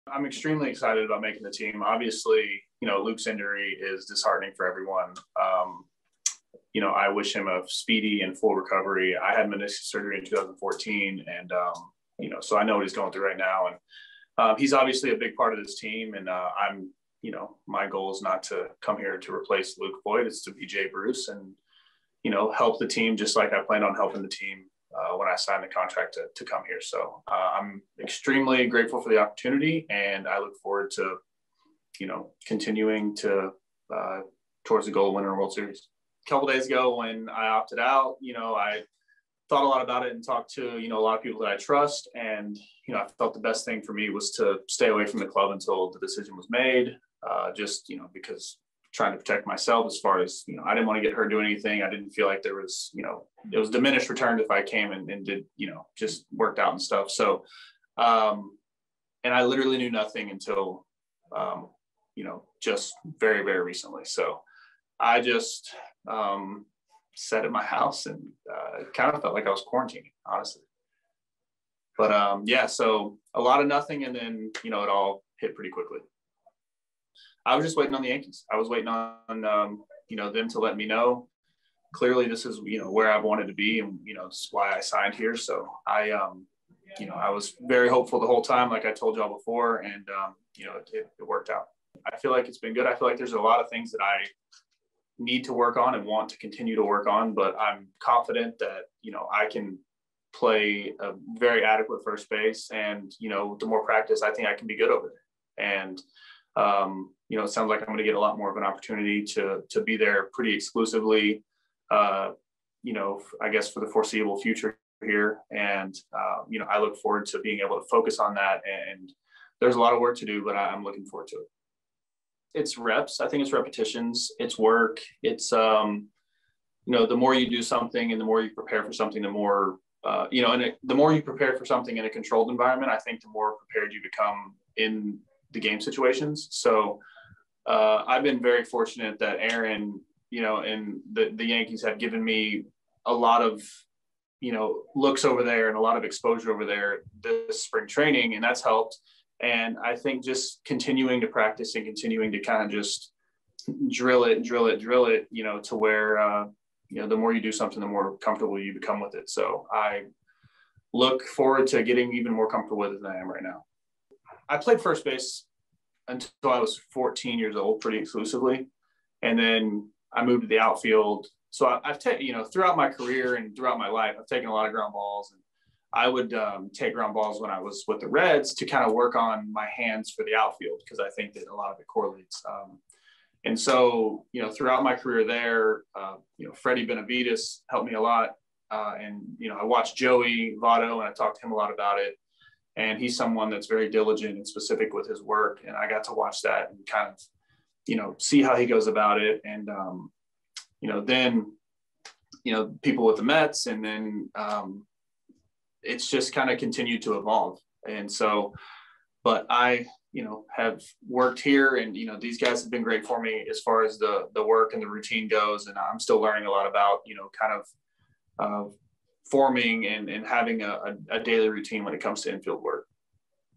Jay Bruce on making the Yankees MLB roster
As Luke Voit prepares to undergo knee surgery, Jay Bruce is excited for the opportunity to open up the season with the Yankees at first base.